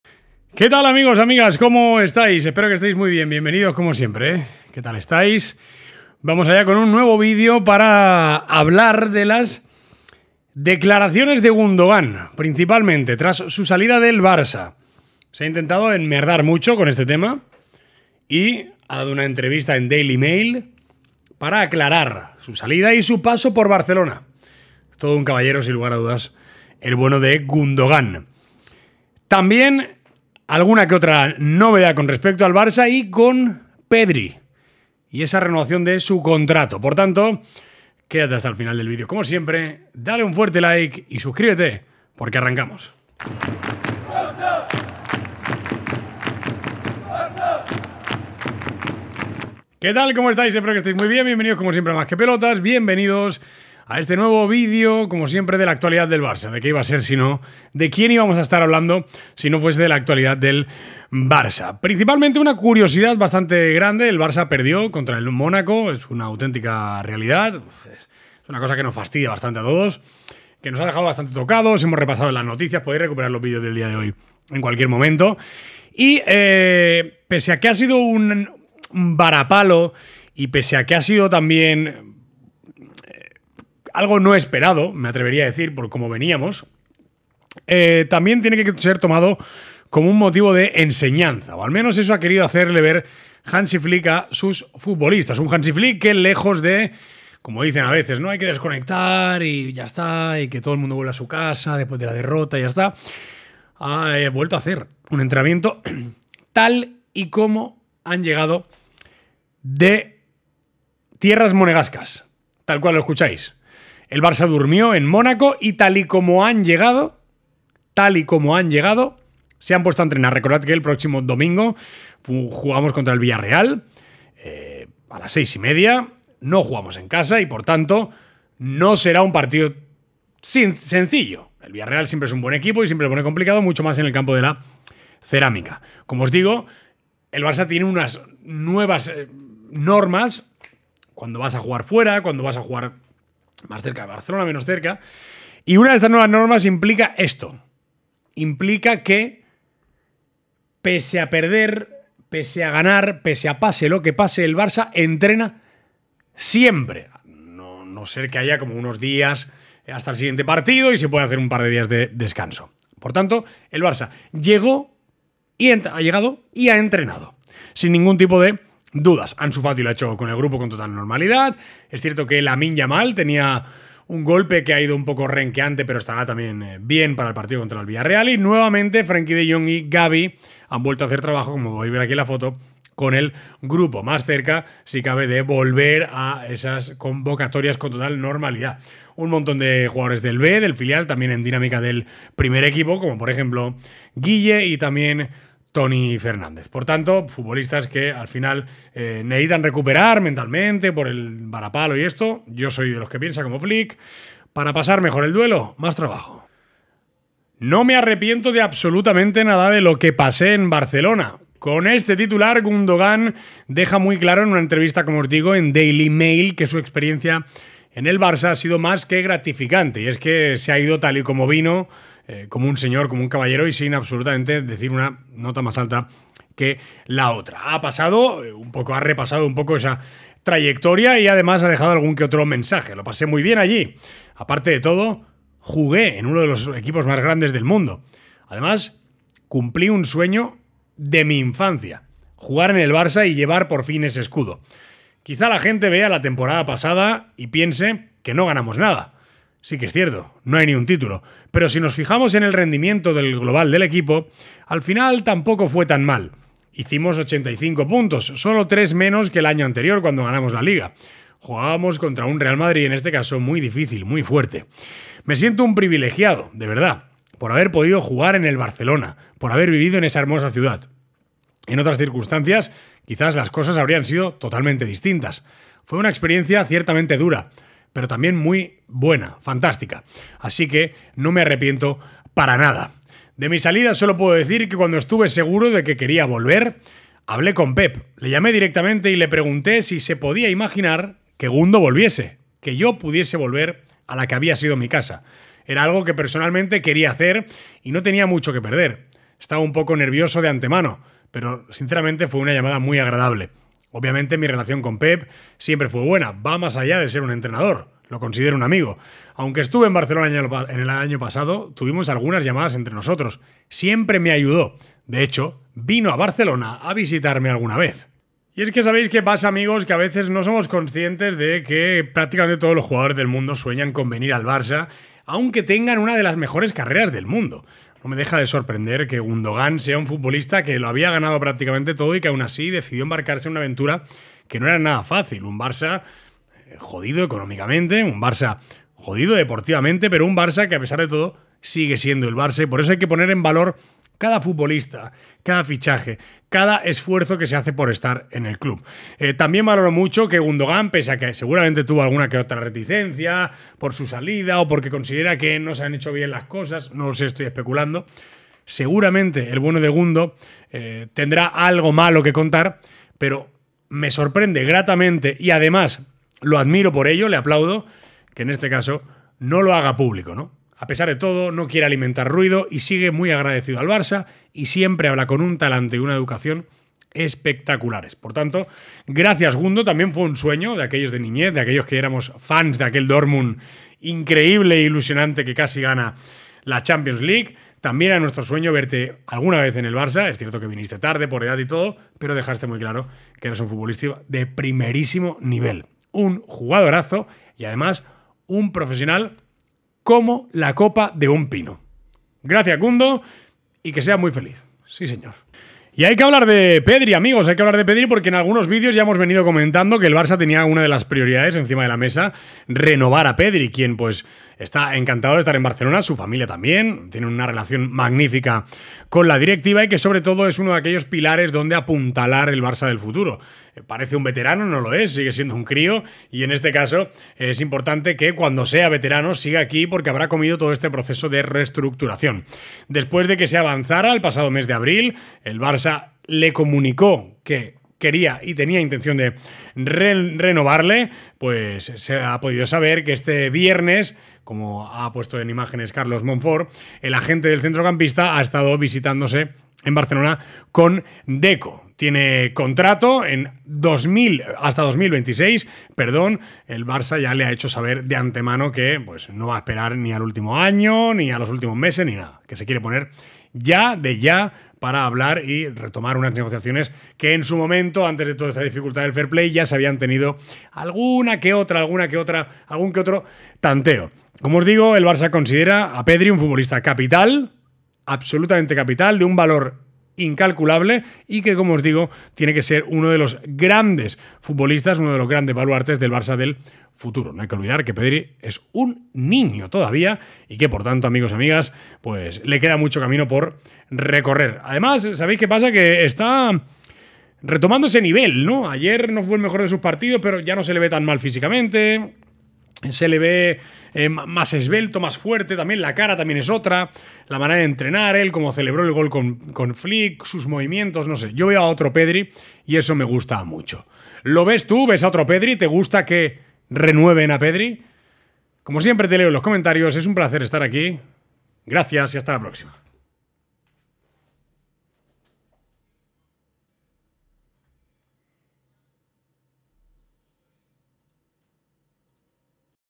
💥ATENCIÓN: NEGOCIACIONES AVANZADAS PARA RENOVAR A PEDRI | GUNDOGAN LO ACLARA TODO
En este video, te traemos todas las noticias sobre las negociaciones avanzadas para renovar a Pedri y lo que Gündogan tiene que decir al respecto de su paso por el Barça. ¡No te lo pierdas! ¡Mantente informado con las últimas novedades!